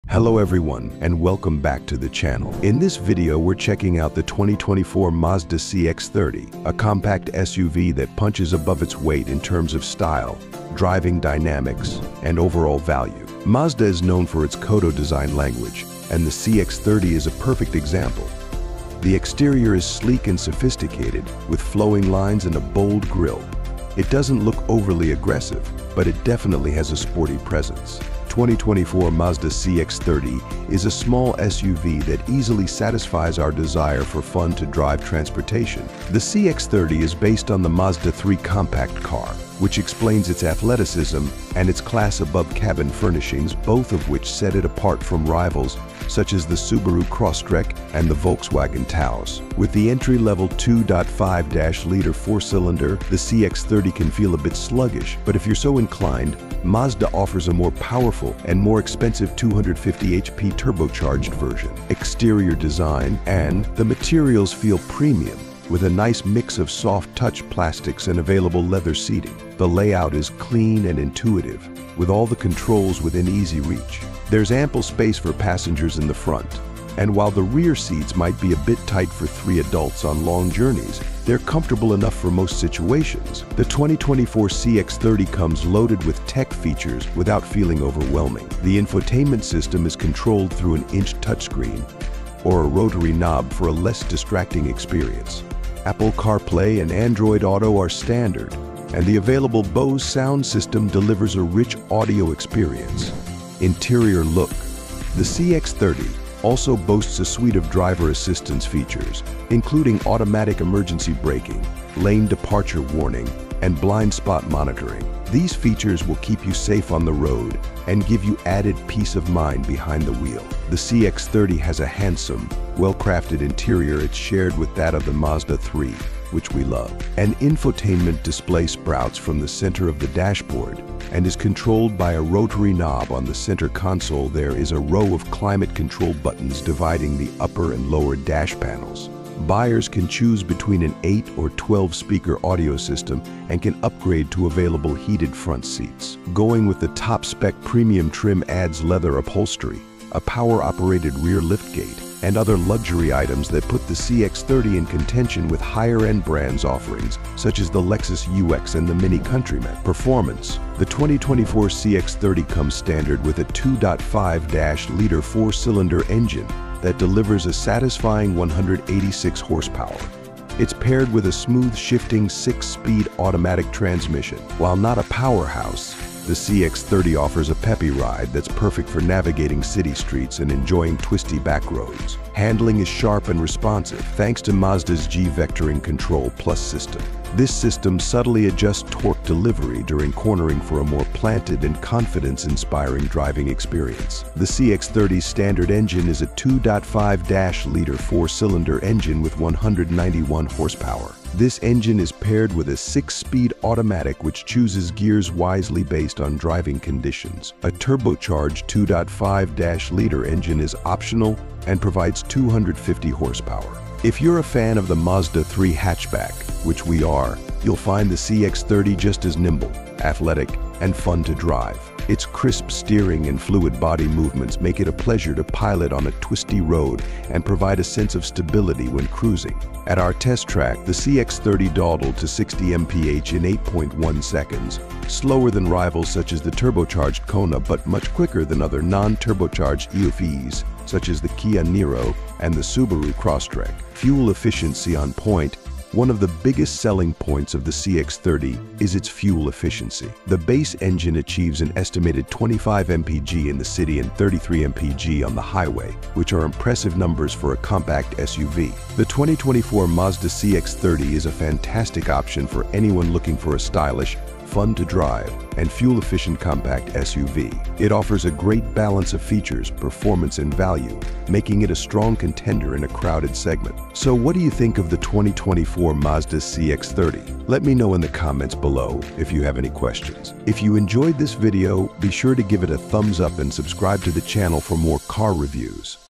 Mazda CX-30 NEW 2024 - RESTYLED Crossover SUV
Discover the 2024 Mazda CX-30 in our detailed review! Explore its stunning design, advanced technology, and performance features that make it stand out in the compact SUV market. Whether you're considering a new ride or just a car enthusiast, this video covers everything you need to know about the 2024 Mazda CX-30